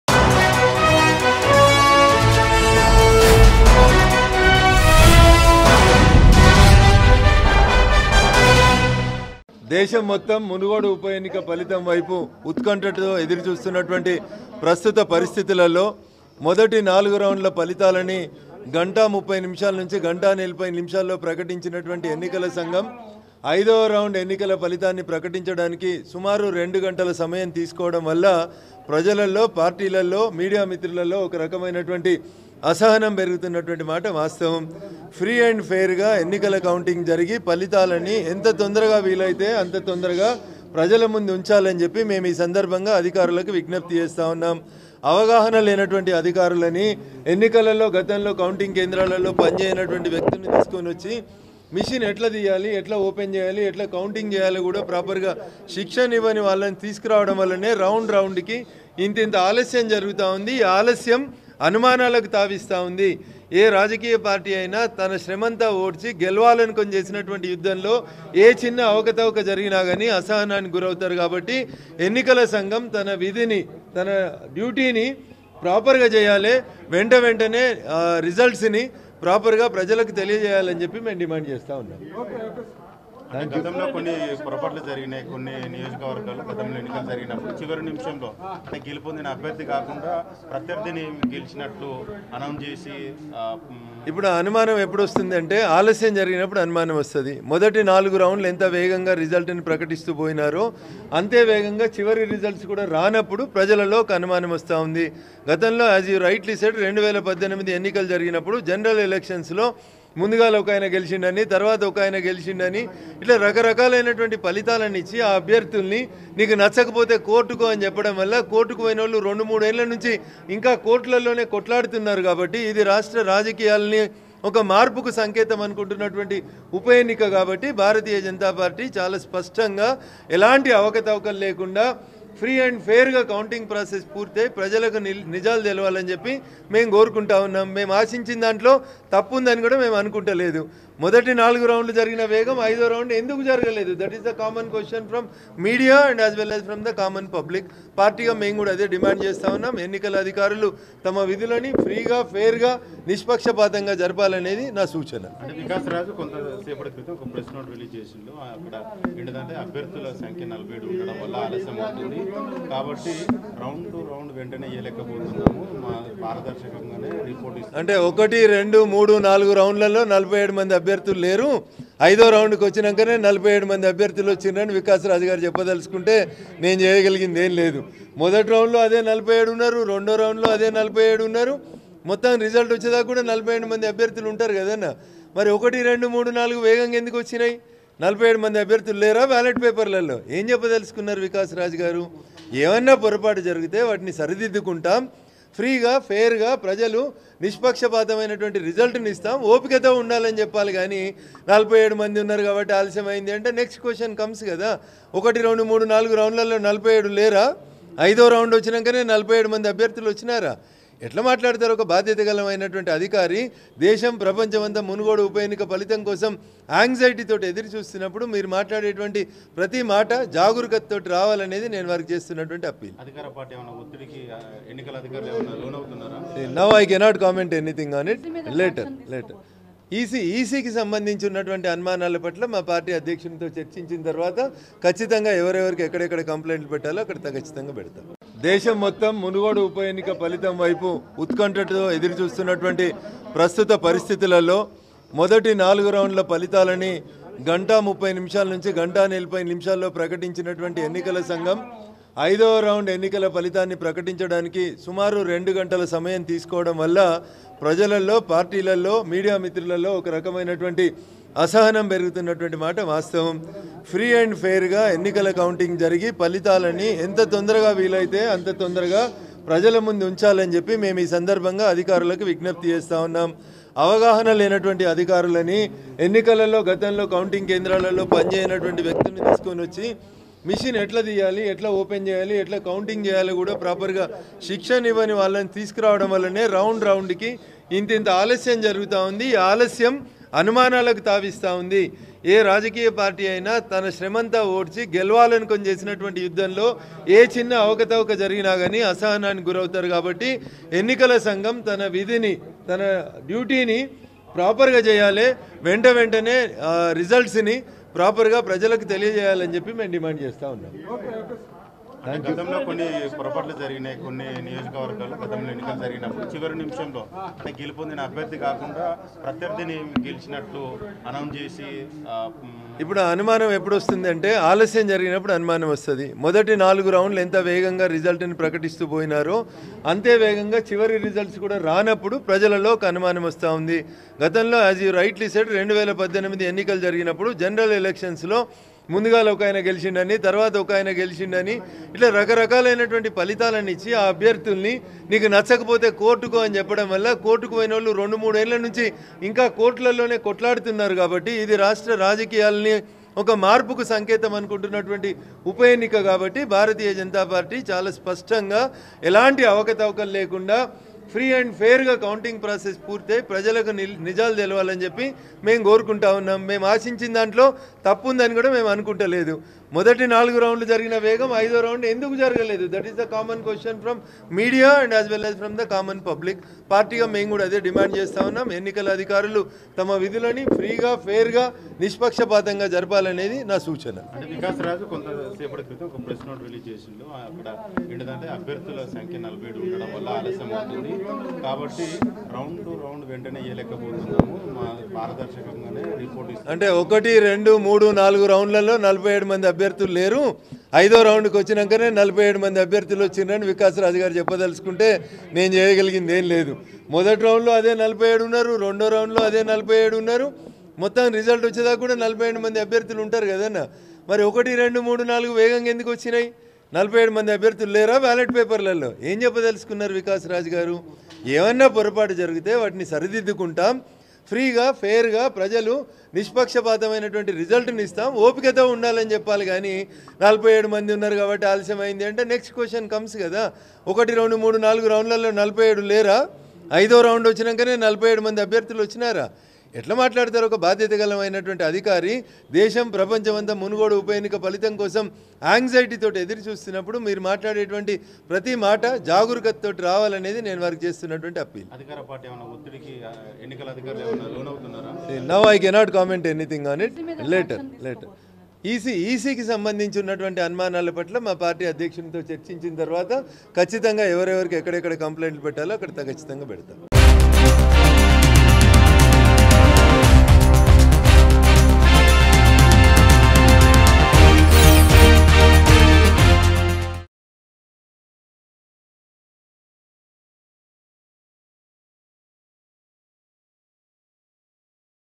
MLA Raghunandan Rao Serious Over Delay Of Munugodu By-Election Results | Munugode | Mango News
MLA Raghunandan Rao Serious Over Delay Of Munugodu By-Election Results.